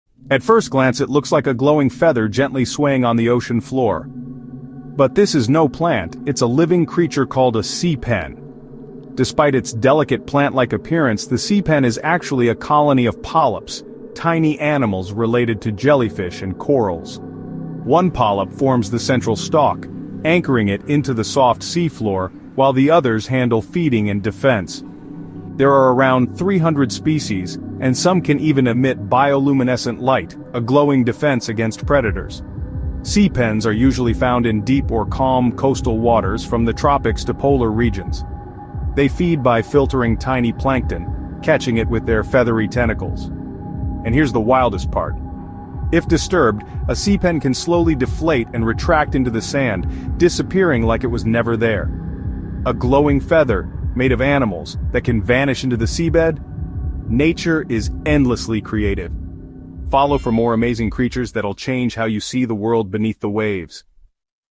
The Glowing Feather That’s Actually Alive: Meet the Sea Pen
It looks like a glowing underwater feather… but it’s alive 😳
Meet the Sea Pen—a colony of animals that lights up and hides in the sand.